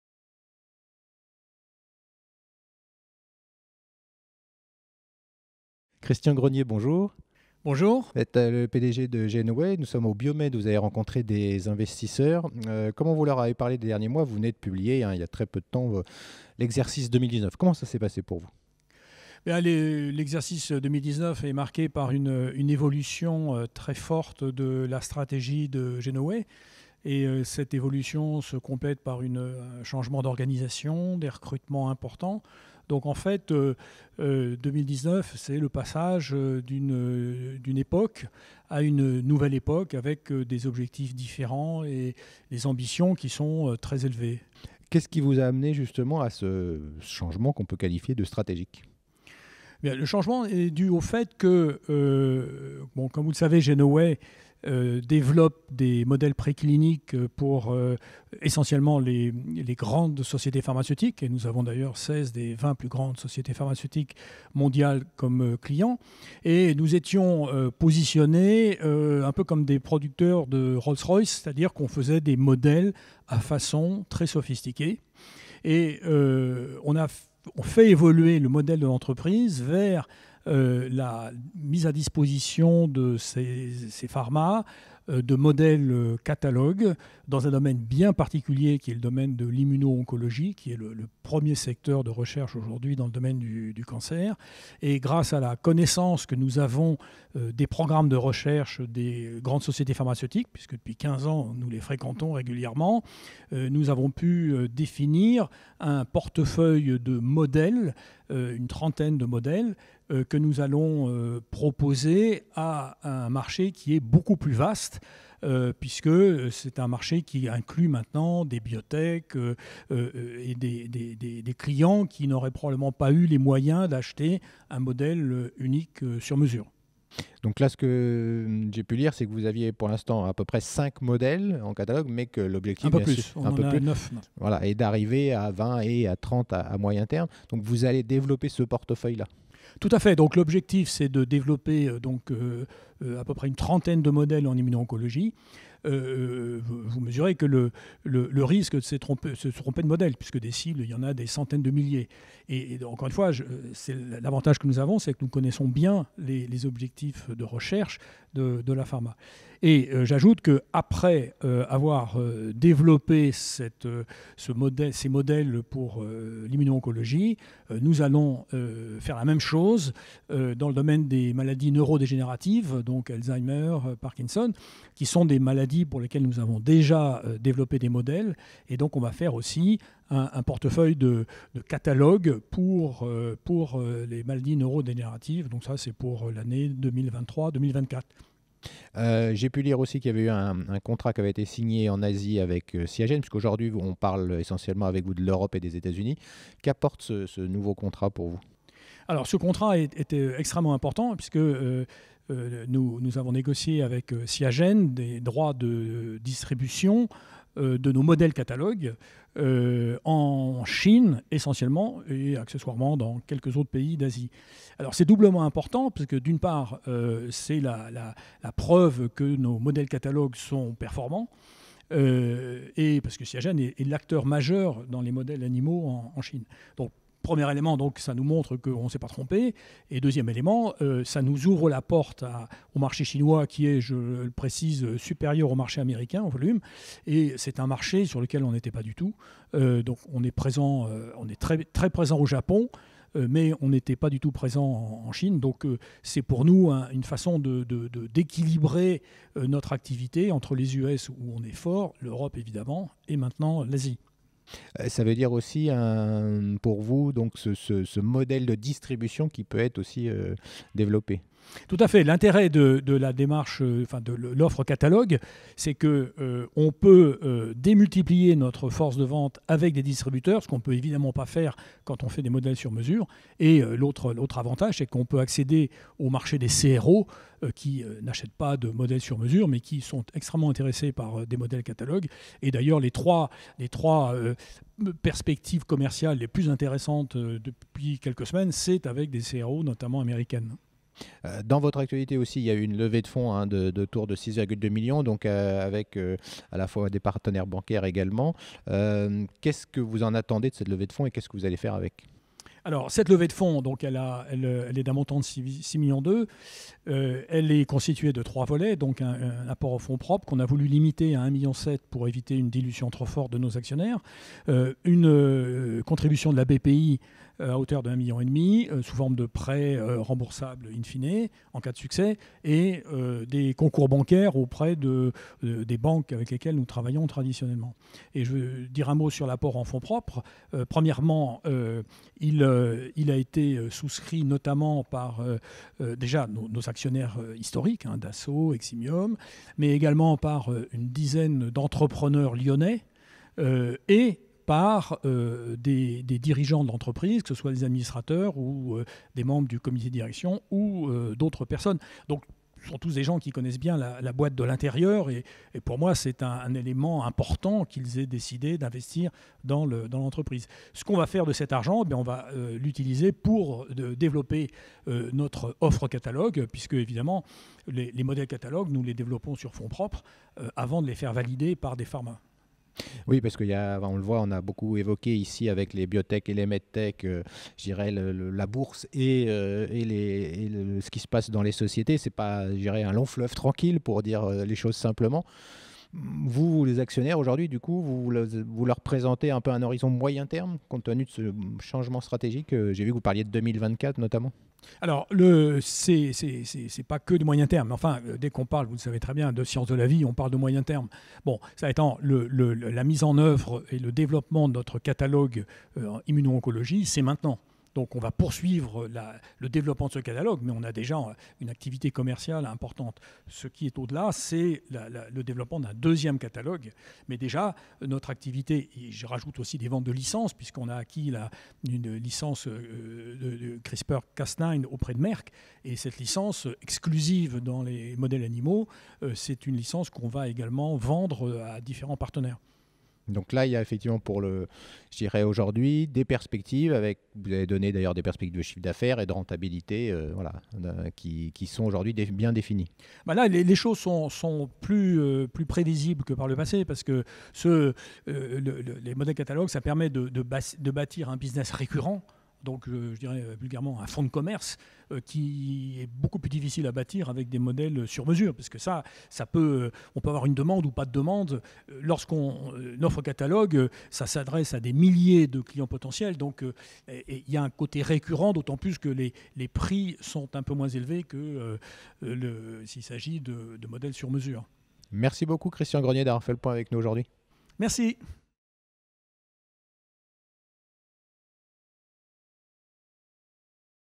Interview Christian Grenier Pdg genOway par Didier Testot
GenOway, société de biotechnologie leader dans la conception et le développement de modèles de recherche génétiquement modifiés et Merck, société scientifique et technologique leader dans la modification génétique, ont annoncé récemment avoir conclu une alliance stratégique dans le domaine des modèles CRISPR/Cas9 pour le marché des modèles de rongeurs.
Elle vient d'annoncer une augmentation de capital pour déployer un plan stratégique dont je parle avec Christian Grenier Pdg genOway.
La société est une entreprise de services pour les sociétés de biotechnologies et pharmaceutiques.